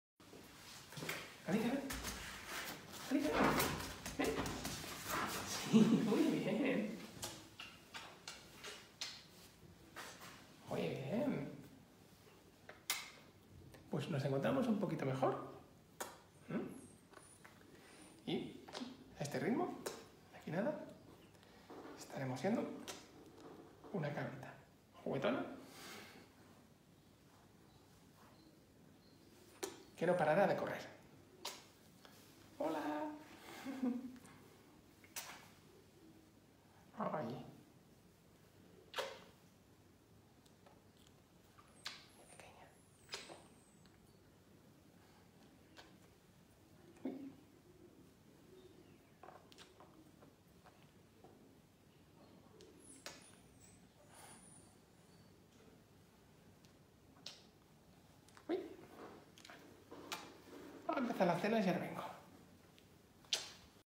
Galicia se sube en brazo de su papi
"Me encanta cuando mi papi pasa por mi cunita antes de ir fuera a trabajar y me coge y me da un abrazo. Luego cuando termine las cenas estaremos juntos hasta mañana por la mañana. ¡Es mi rato favorito del mundo! "